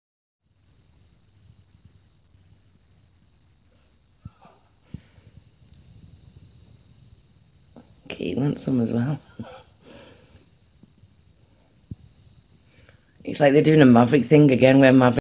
4 June 2016
The posing kitty the puppy and a box off chocolate.